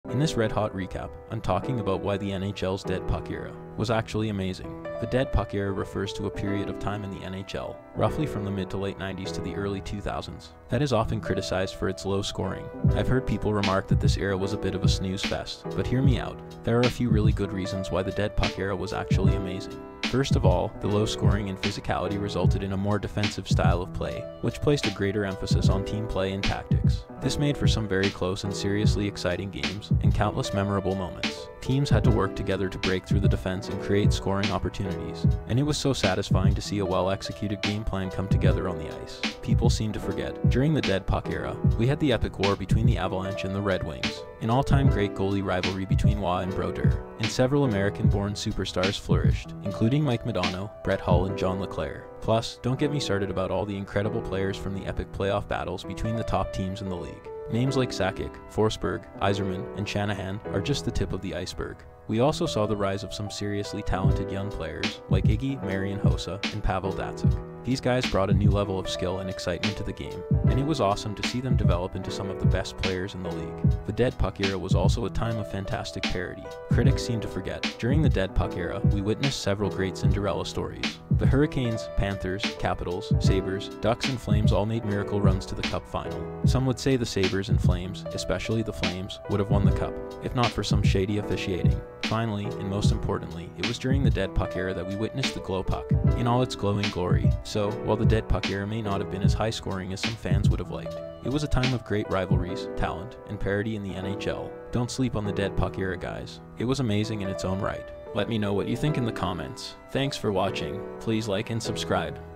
The NHL's dead puck era was actually amazing
In this Red Hot Recap, I reflect on the NHL's so-called "dead puck era" and try to convince (or remind) you that it was actually really fun!